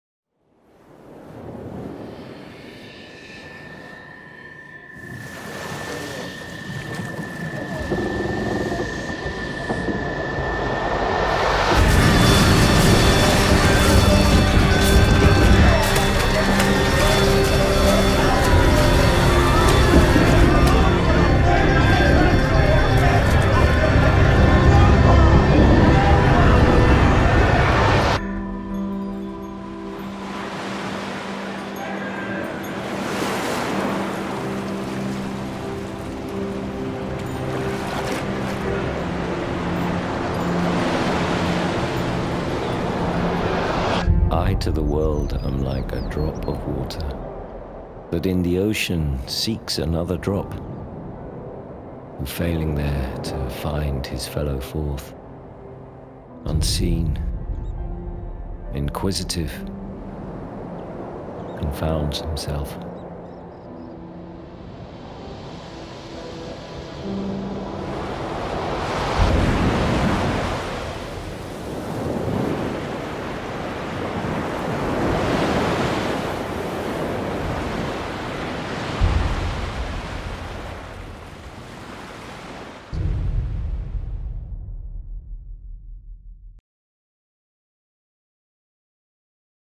What Country Friends Is This? | World Shakespeare Festival | Royal Shakespeare Company
Shakespeare's shipwreck trilogy - from 8 March
Royal Shakespeare Theatre, Stratford-upon-Avon and The Roundhouse, London

Led by RSC Associate Director David Farr, one ensemble company of RSC actors will perform The Comedy of Errors, Twelfth Night and The Tempest, three plays exploring love, loss and reunion.

The three plays date from the early, middle and late periods of Shakespeare's work as he returned throughout his life to further explore these themes. While each production can be enjoyed on its own, seen together they are an epic trilogy by the world's greatest dramatist exploring issues that are as relevant today as they were 400 years ago.

The company includes Jonathan Slinger as Malvolio and Prospero, having recently played Macbeth in the first production in the transformed Royal Shakespeare Theatre. The productions open in Stratford-upon-Avon at the Royal Shakespeare Theatre from March, transfering to London's Roundhouse in June, before returning to Stratford-upon-Avon in July.